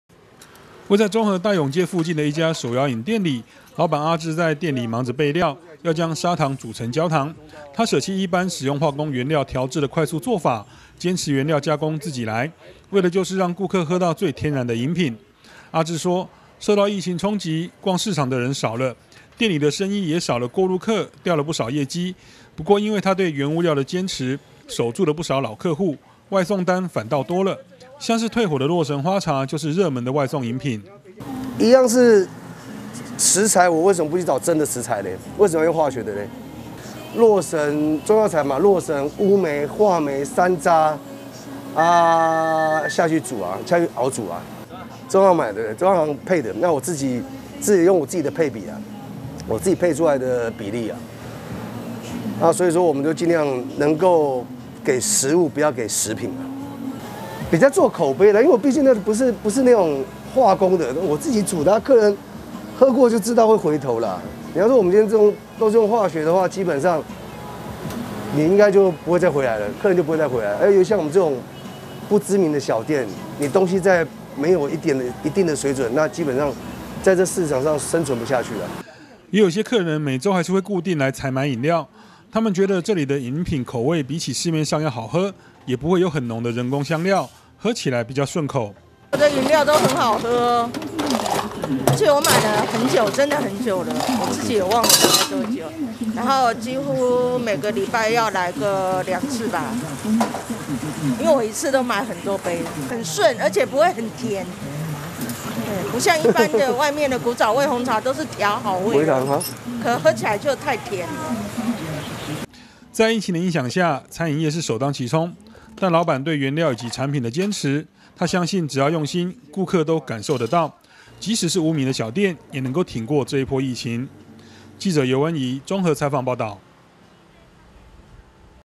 疫情 餐飲|無懼疫情衝擊 飲料店老闆堅持手作原料
在新冠疫情的衝擊下，有不少行業都受到影響，在中和大勇街黃昏市場附近，有家小型的手搖飲店，老闆堅持不用化工原料，自己親手熬煮，得到死忠老客戶的支持。